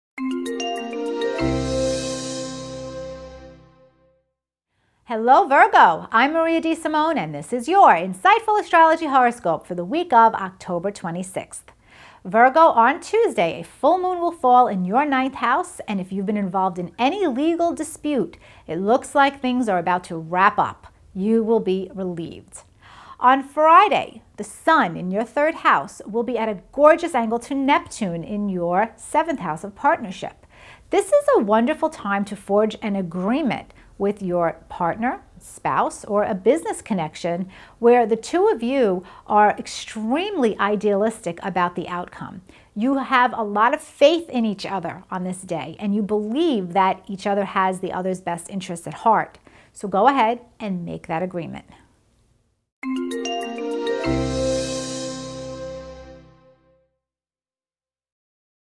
Virgo Week of October 26th 2015 Horoscope (*October Horoscope*)
Virgo: August 24th – September 22nd
Insightful Astrologer Maria DeSimone presents your weekly horoscope for the Week of October 26th for the sign: Virgo